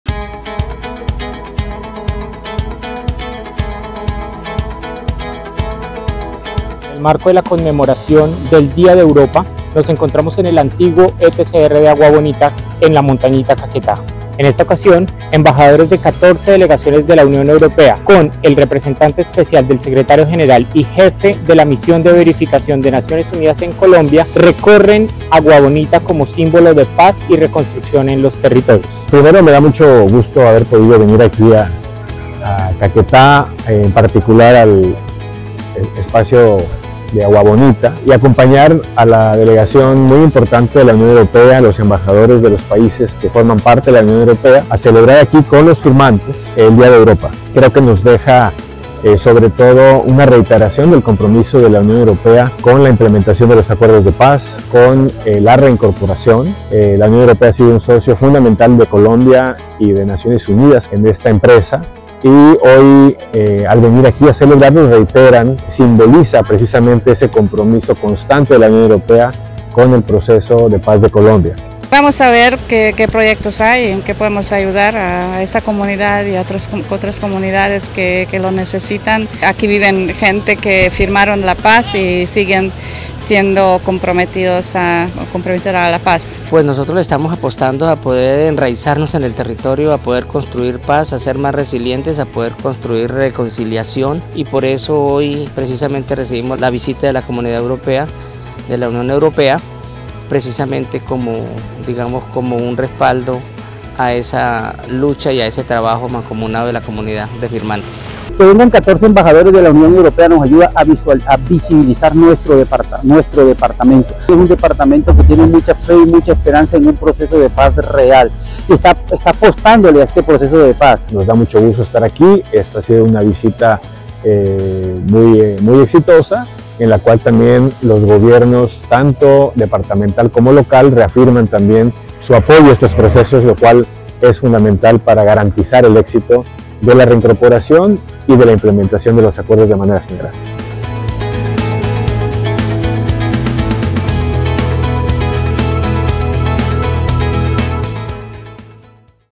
Día de Europa en Agua Bonita, Caquetá. Mayo 6, 2024
En el DíaDeEuropa, Carlos Ruiz Massieu, Representante Especial del Secretario General de la ONU en Colombia, junto a los Embajadores y las Embajadoras de la Unión Europea y sus Estados Miembros presentes en Colombia, autoridades departamentales y locales conocieron el espacio donde confluyen la voluntad de las y los firmantes con su reincorporación comunitaria, la presencia de la institucionalidad y el apoyo de la cooperación internacional con el propósito de construir un futuro sostenible y en paz. 🇨🇴🇺🇳🇪🇺